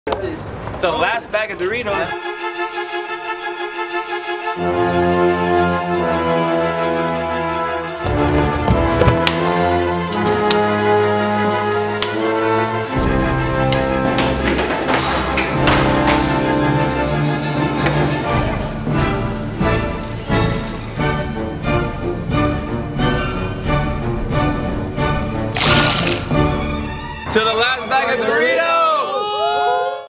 The Heist
The last bag of Doritos on Earth Exhibition is at the City Museum in February 2016. High school students see the poster and decide that they will plan a heist in order to enjoy the last bag of Doritos!